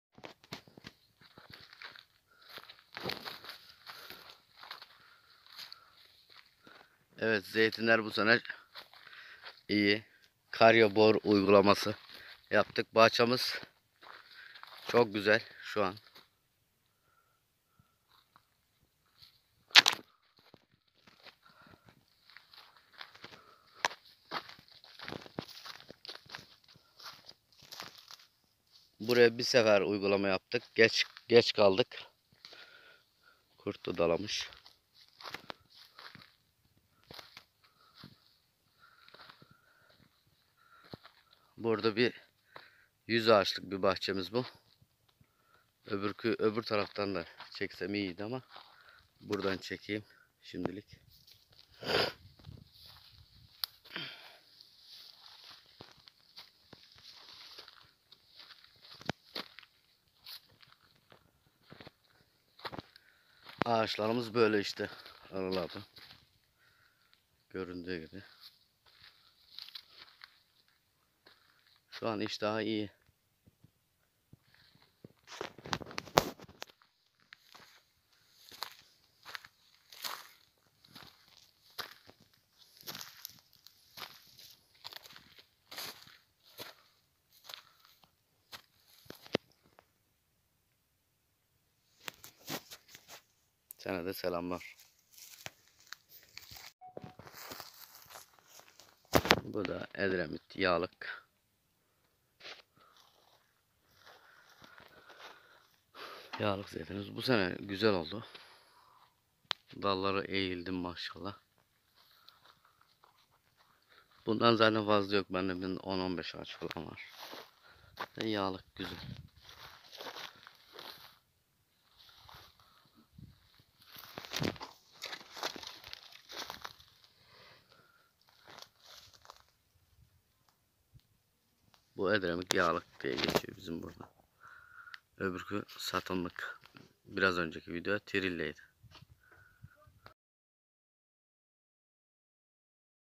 Zeytin'de Tek Uygulama Bile Fark Attı Karia Bor 8 Gübresi İle-Alaşehir'de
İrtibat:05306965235
Yapraktan tek sefer bor ve çinko eksikliği için yapraktan bol mineralli Karia Bor 8 Gübresi ile besleme yapıldı.
Konum:Manisa-Alaşehir-Çarıkbozdağ Mahallesi..